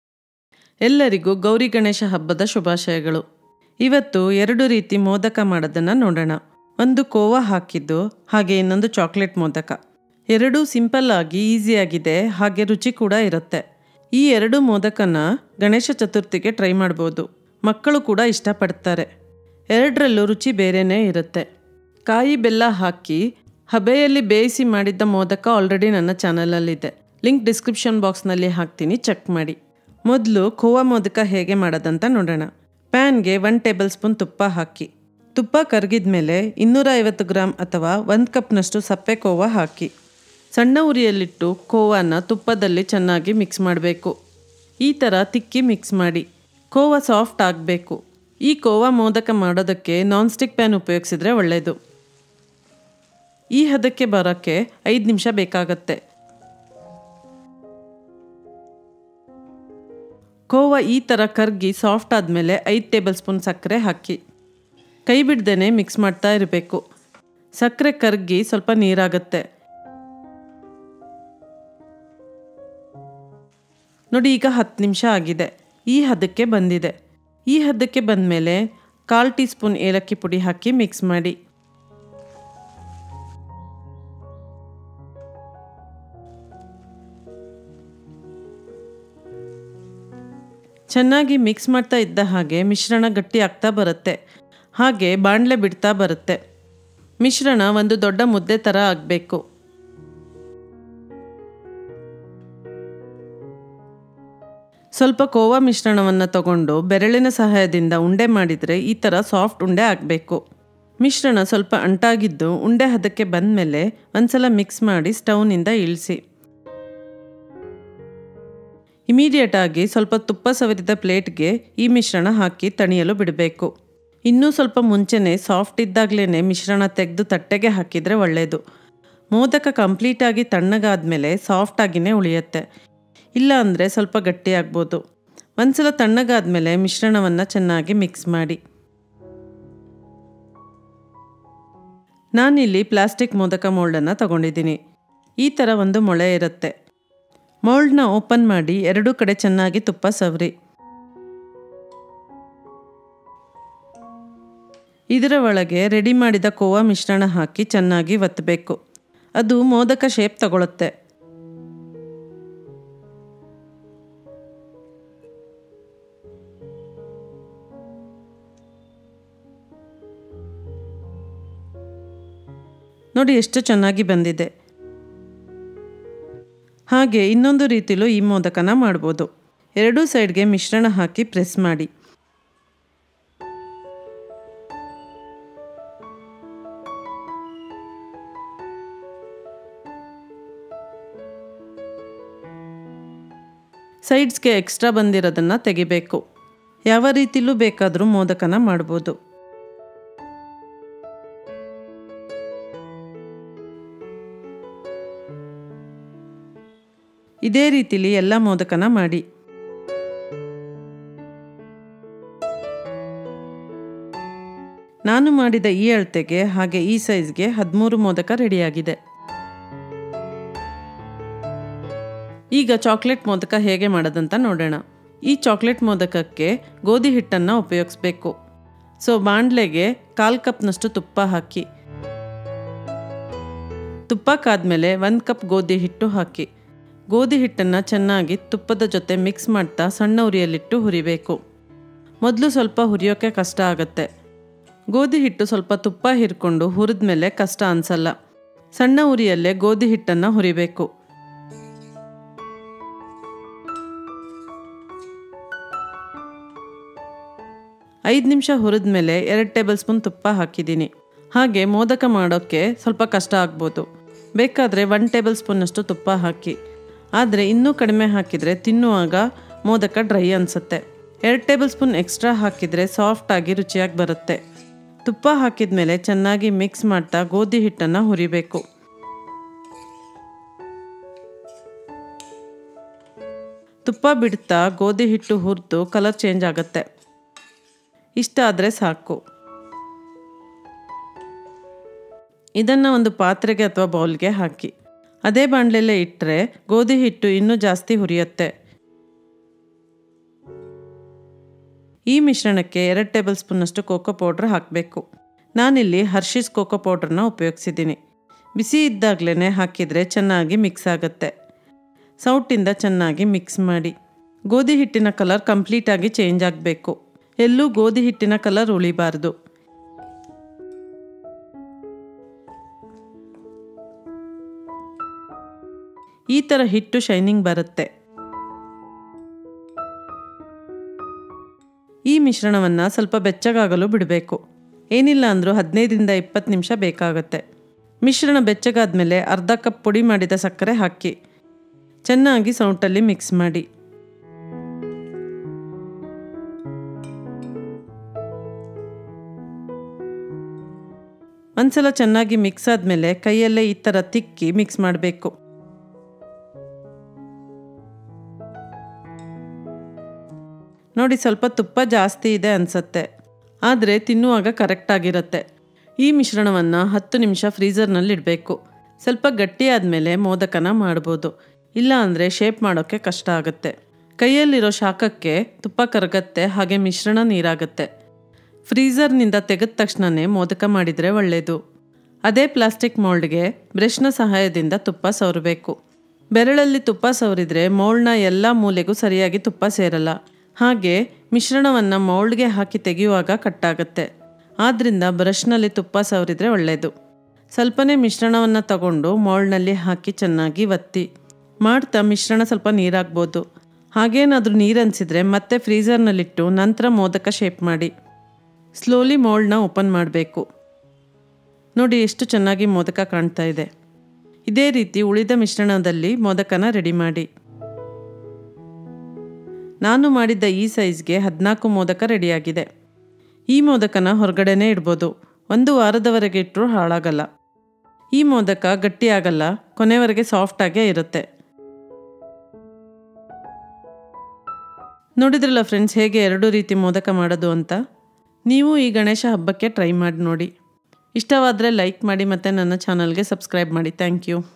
ಗಣೇಶ ಚತುರ್ಥಿಗೆ ಮೋದಕ | Easy Modak Recipe for Ganesh Chaturthi
This Ganesh Chaturthi, prepare these two varieties of Modaka or Modak - Chocolate Modak and Peda Modaka. Narrated in Kannada. English subtitles are coming soon.